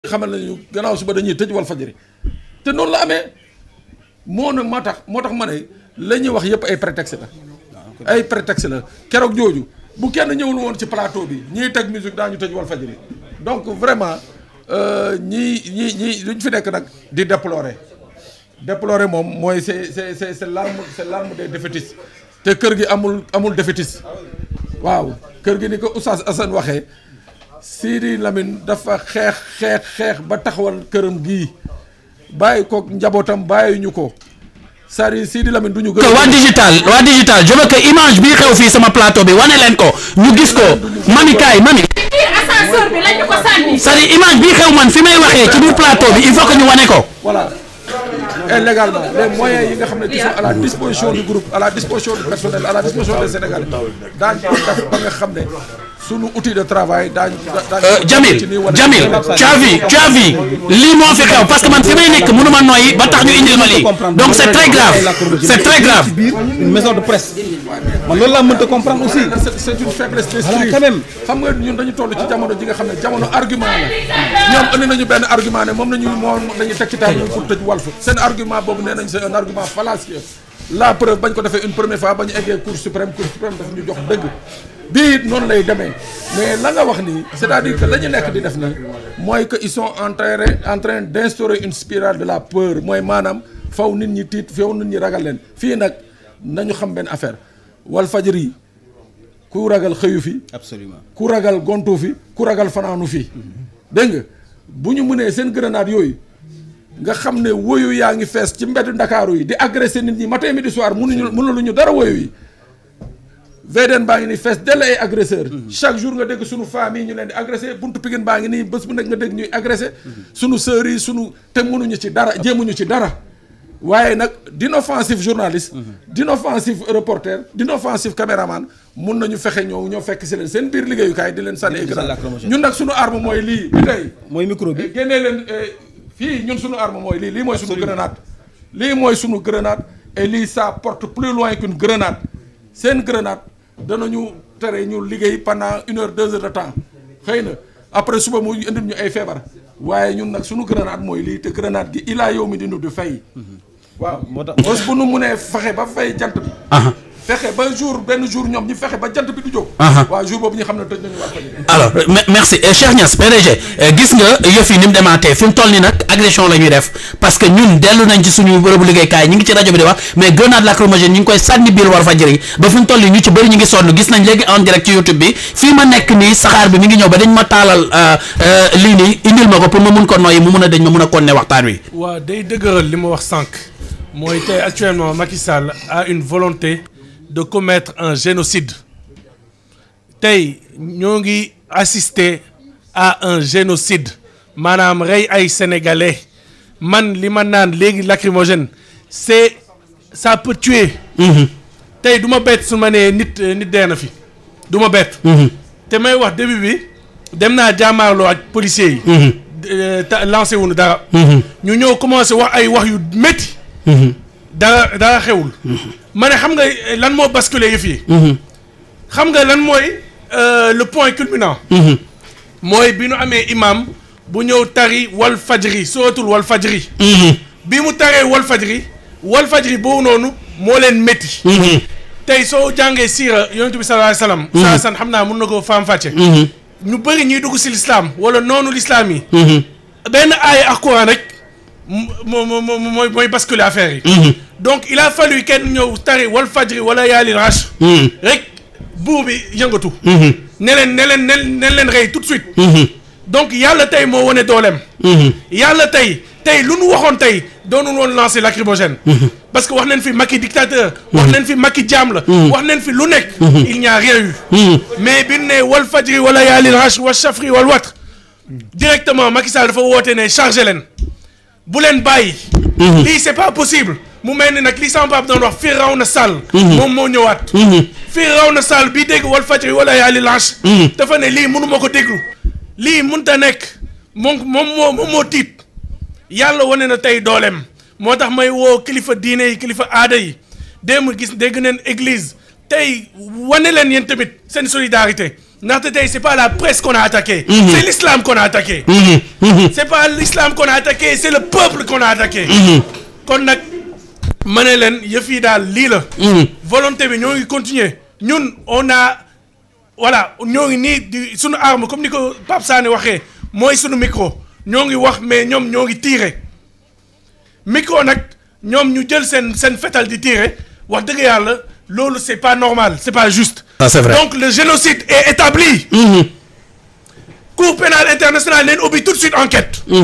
Cheikh Niass "Ce qui se passe à Walf c'est un génocide, la cour pénale internationale doit réagir
Edition Spéciale suite au gaz lacrymogène tirés aux locaux de Walfadjri